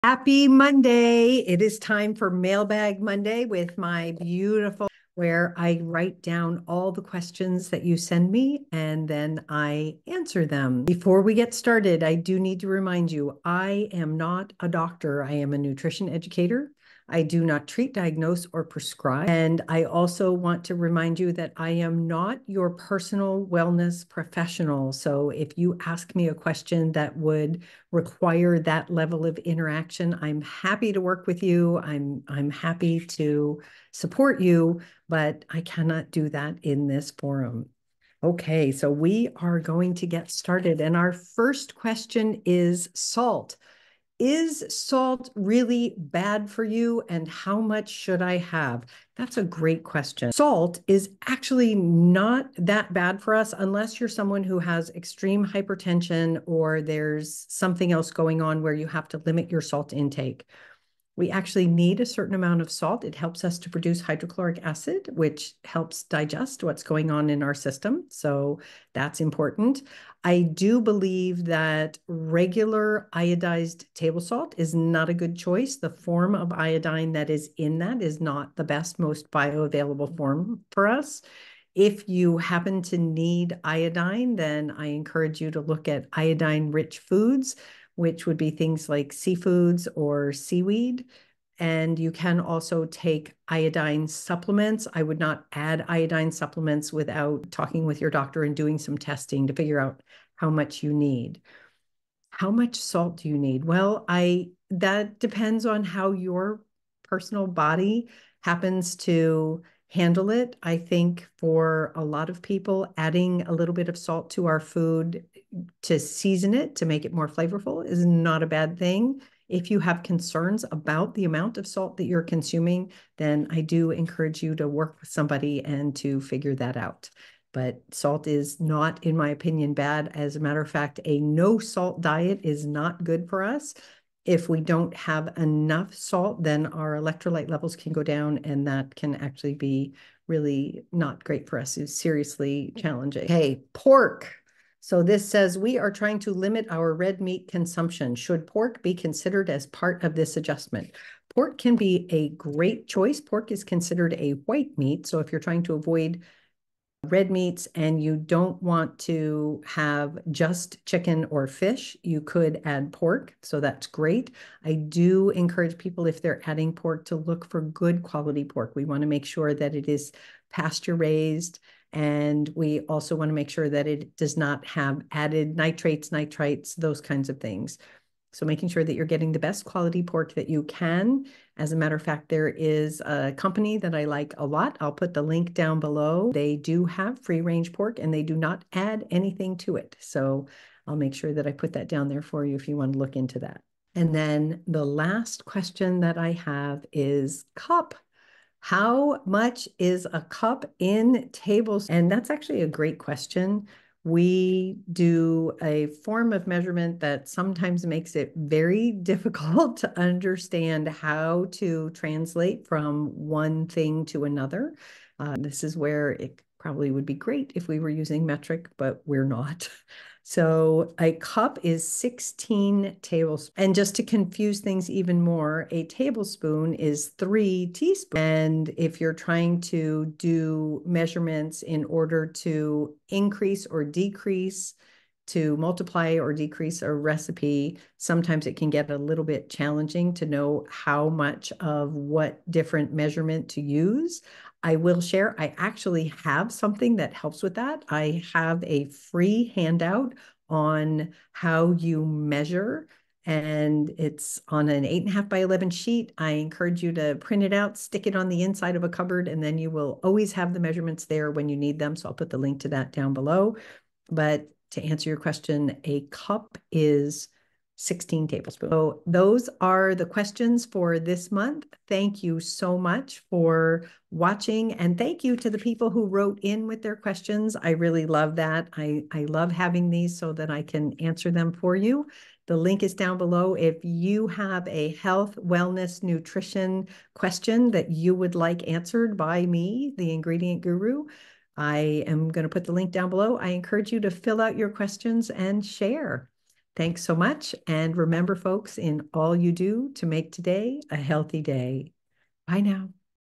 Mailbag Monday #9: Salt Intake, Pork Consumption, and Measurement | Mira Dessy, The Ingredient Guru
In this episode of Mailbag Monday, Mira discusses whether salt is bad for you, how much salt you should consume, and the importance of choosing the right type of salt. The Ingredient Guru also talked about incorporating pork into your diet while limiting red meat, and provide tips on selecting high-quality pork. Additionally, she covered measurement conversions, offering a free hand offering a free handout for easy reference.

Submit your health and nutrition questions for future sessions and remember to make today a healthy day!

Time tags:

00:47 - Discussion on salt intake
01:10 - Importance of salt and alternatives
01:38 - Considerations for salt intake
02:07 - Discussion on pork consumption
02:45 - Quality considerations for pork
03:08 - Recommendation for a pork supplier
03:24 - Explanation of measurement: cup to tablespoons
04:23 - Handout on measurements

Connect With Mira Dessy, NE, The Ingredient Guru

TheIngredientGuru
MiraDessy